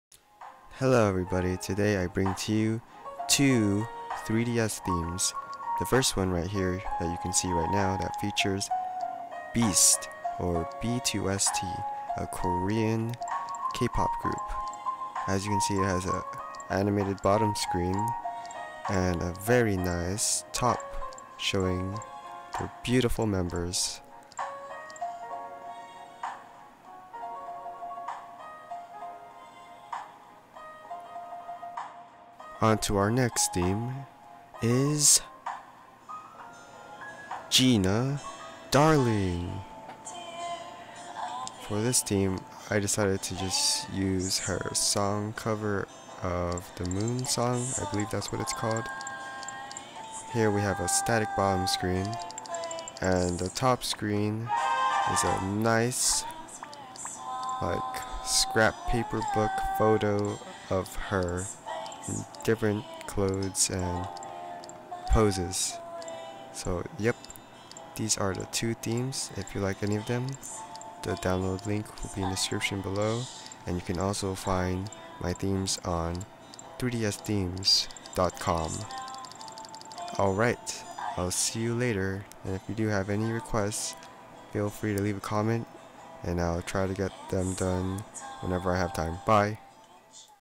[3DS] Pack of 2 3DS Custom Theme - B2ST, Gina Darling + Free Downloads
Hello, this was a request. My next theme is a special one :D
Also, if you are using ver. 10.1. + US on 3ds and TubeHax does not work. Re-do everything. Make sure you delete YouTube app and do not create back up data. Then it should work again after a reset.